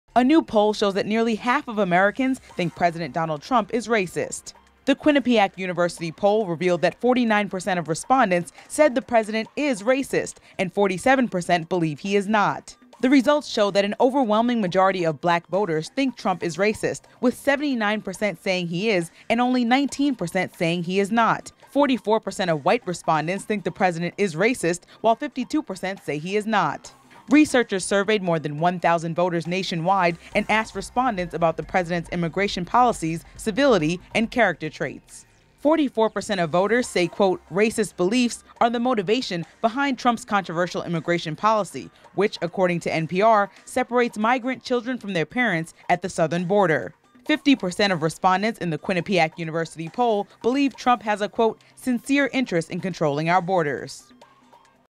Almost Half of Americans Think President Trump is Racist
The Quinnipiac University poll revealed that 49-percent of respondents said the president is racist and 47-percent believe he is not.